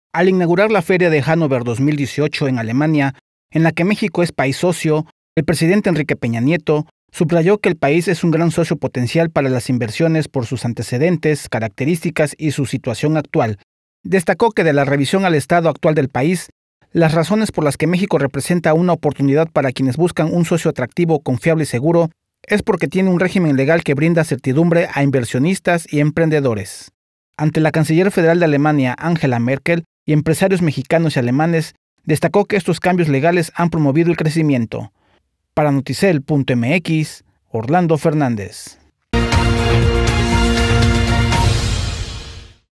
México, gran socio de Alemania
Principalmente para las inversiones, dice Peña Nieto en la inauguración de la feria Hannover 2018